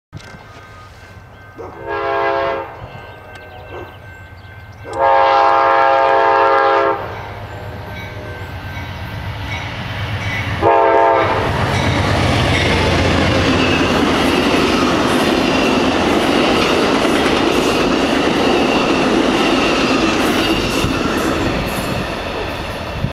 Crowley, Texas - Amtrak 22 "Texas Eagle" - 04/16/2012
Engine 124 provides the power for the northbound Texas Eagle.  This video was taken at about MP 333.20 of the BNSF Fort Worth Sub, in Crowley, Texas.